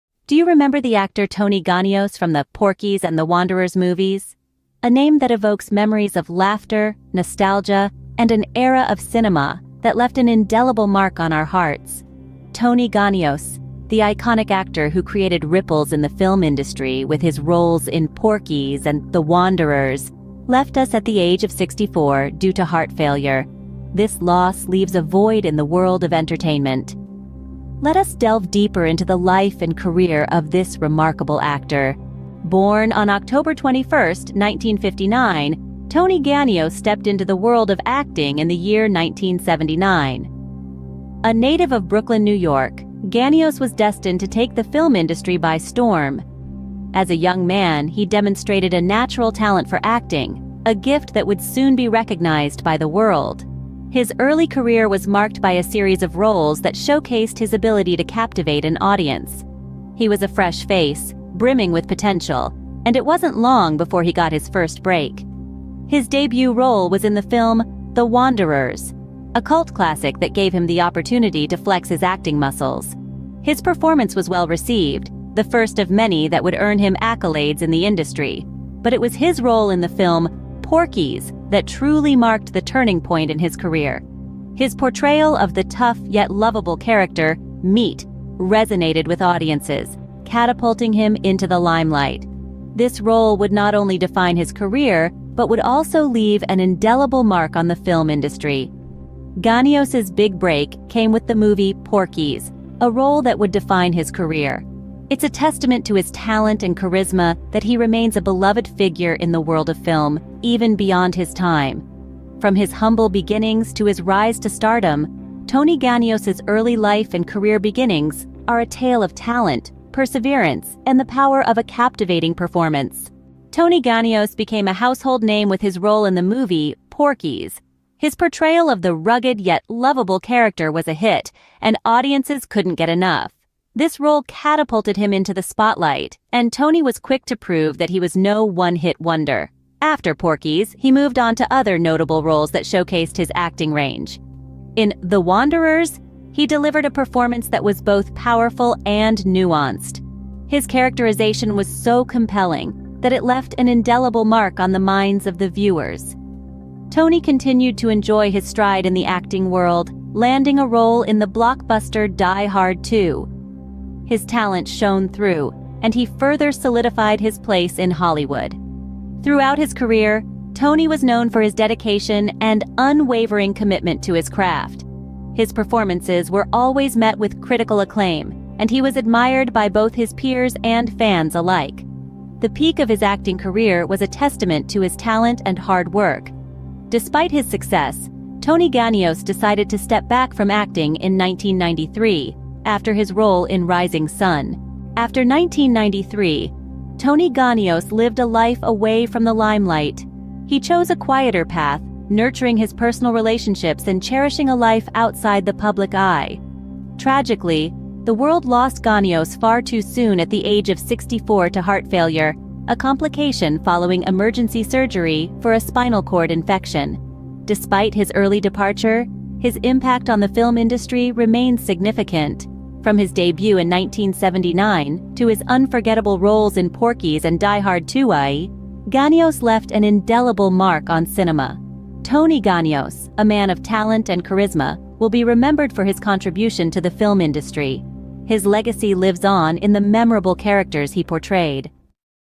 Remembering Tony Ganios: A Tribute to a Film Legend
In this heartfelt tribute video, we honor the memory of the late Tony Ganios, a truly beloved actor known for his iconic roles in 'The Wanderers' and 'Porky's'. Join us as we reminisce about his remarkable career and enduring impact on the film industry. Let's celebrate the life and talent of Tony Ganios together.